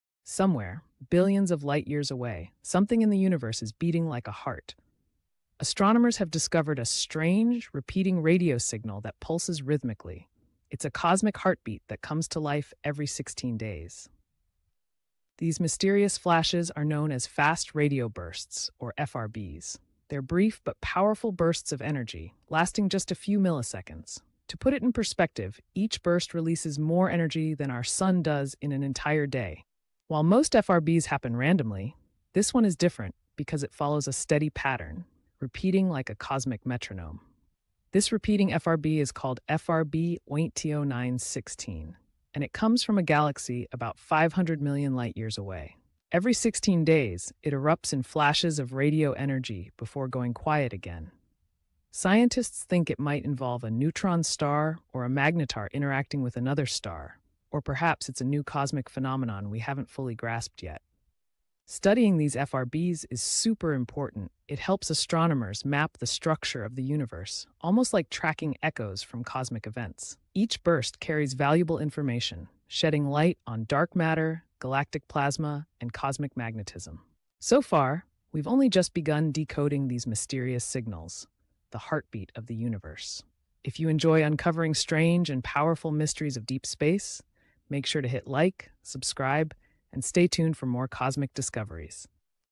The Mysterious Heartbeat from a Distant Galaxy 💓 | The Cosmic Radio Signal Scientists Can’t Explain
Somewhere in the vast universe, scientists have detected a cosmic heartbeat — a repeating radio signal pulsing every 16 days from billions of light-years away. Known as a Fast Radio Burst (FRB), this mysterious phenomenon releases more energy in a millisecond than the Sun emits in an entire day.

In this video, we’ll explore:
✨ What FRBs are and how they’re detected
🌀 Why this signal repeats like a “heartbeat”
💫 What scientists believe might be causing it
🌌 And what this tells us about the structure of our universe

Join us as we uncover one of the most mysterious signals ever discovered in space — a heartbeat echoing through the cosmos. 💓